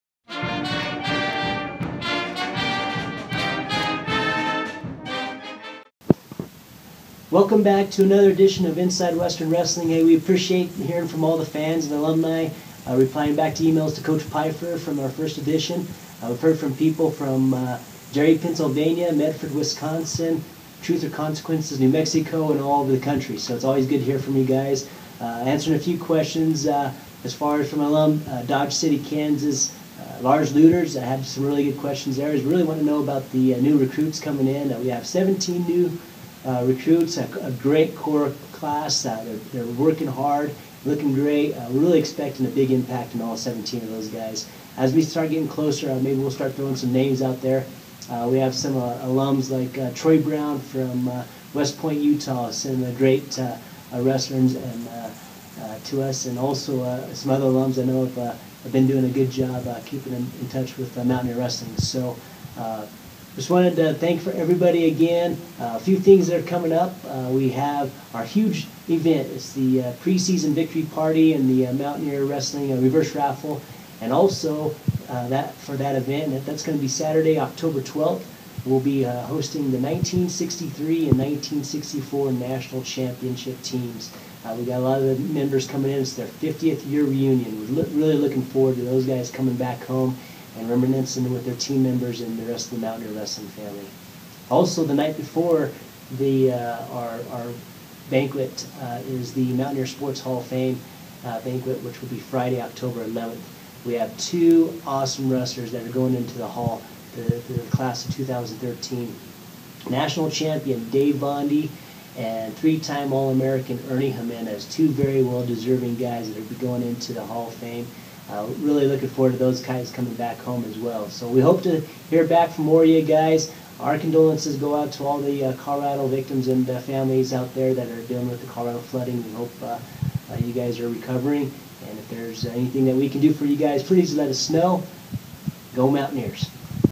Inside Western Wrestling | Sept. 18, 2013
Western State Head Wrestling Coach Miles Van Hee fields questions from Mountaineer Nation.  In this segment, Coach Van Hee talks about incoming freshmen and the annual Reverse Raffle.